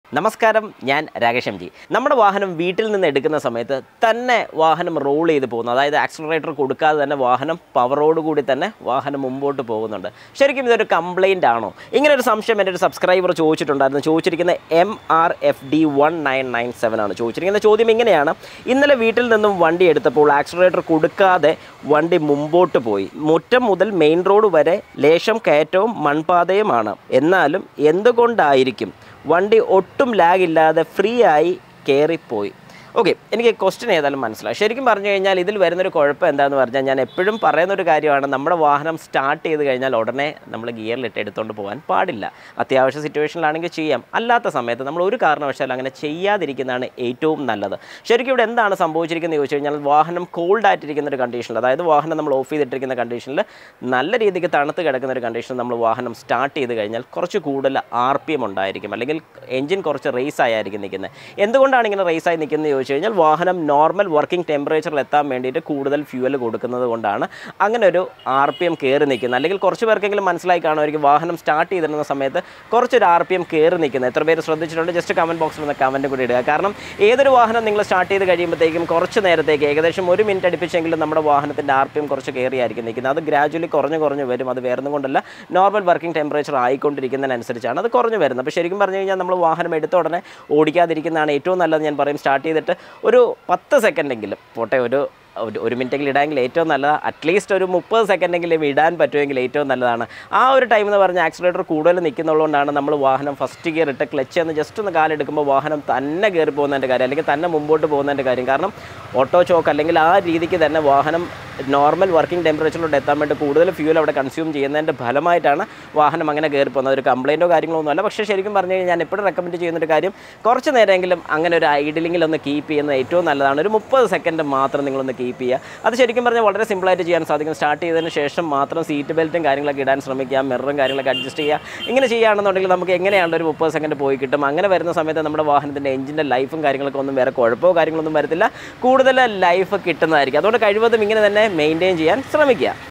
ഇങ്ങനെ ചെയ്യരുത്‌ എഞ്ചിന്‍ കംപ്ലേയിന്റ് വരാം | Vehicle climbing steep without acceleration
Why do the vehicle climb a steep with out even applying the accelarator pedal ? why this happens lets check this out and do share this with all your friends too